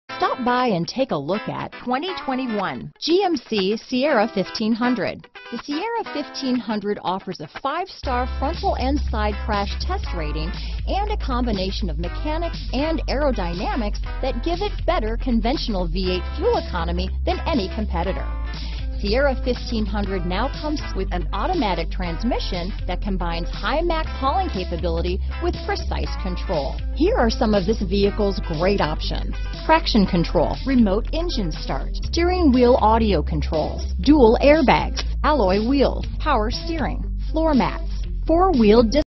2021 GMC Sierra 1500 Carrollton, Franklin, Atlanta, Marietta, Newnan, GA MG390
Summit White New 2021 GMC Sierra 1500 available in Carrollton, Georgia at John Thornton Cadillac Buick GMC. Servicing the Franklin, Atlanta, Marietta, Newnan, GA.
2021 GMC Sierra 1500 SLE - Stock#: MG390 - VIN#: 3GTU9BED2MG194014